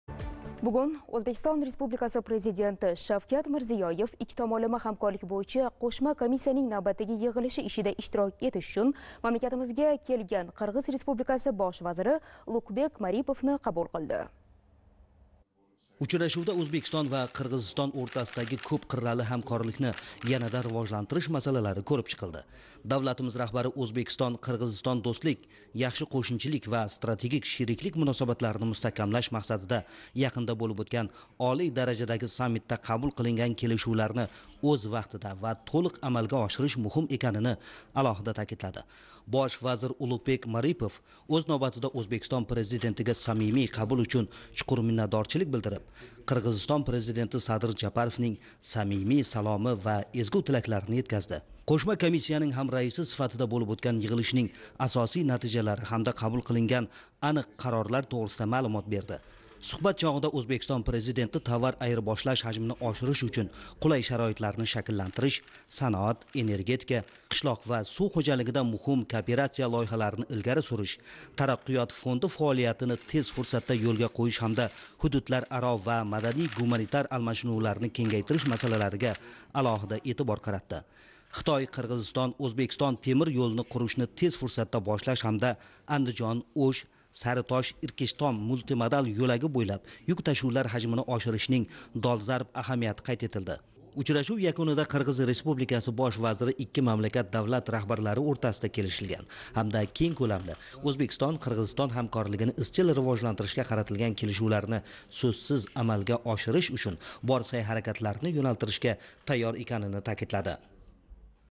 O‘zbekiston Prezidenti Qirg‘iziston Bosh vazirini qabul qildi
O‘zbekiston Respublikasi Prezidenti Shavkat Mirziyoyev 2021-yil 26-mart kuni Ikki tomonlama hamkorlik bo‘yicha qo‘shma komissiyaning navbatdagi yig‘ilishi ishida ishtirok etish uchun Toshkentda bo‘lib turgan Qirg‘iz Respublikasi Bosh vaziri Ulukbek Maripovni qabul qildi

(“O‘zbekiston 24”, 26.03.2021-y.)